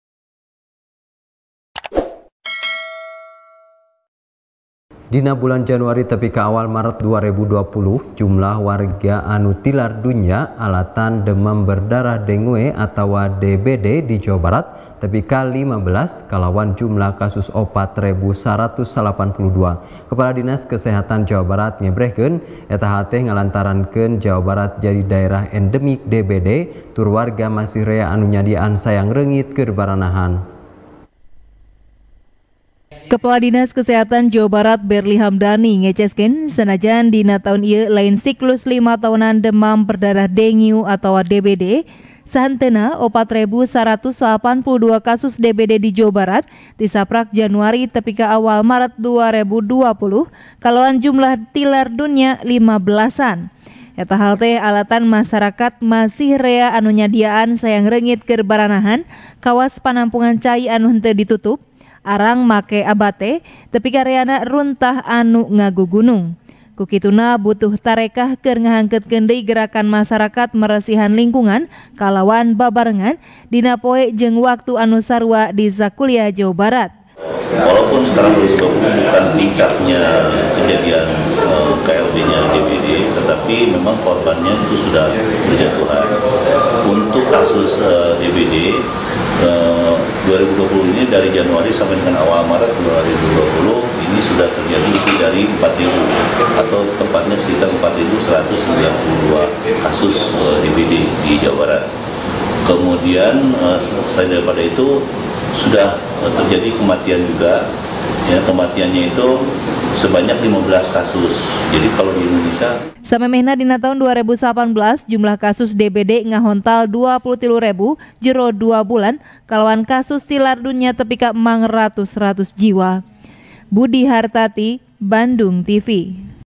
Awal Taun 2020, Warga Lima Belasan Tilar Dunya Alatan DBD Di Jabar | TP | BANDUNGTV
TP | BANDUNGTV | IWAN M.RIDWAN | Dina Bulan Januari Tepi Ka Awal Maret 2020, Jumlah Warga Anu Tilar Dunya Alatan Demam Berdarah Dengue Atawa DBD Di Jawa Barat, Tepi Ka Lima Belas, Kalawan Jumlah Kasus 4 Rebu 192. Kepala Dinas Kesehatan Jawa Barat Ngebrehkeun, Eta Hal Teh Ngalantarankeun Jawa Barat Jadi Daerah Endemik DBD, Tur Warga Masih Rea Anu Nyadiaan Sayang Reungit Keur Baranahan.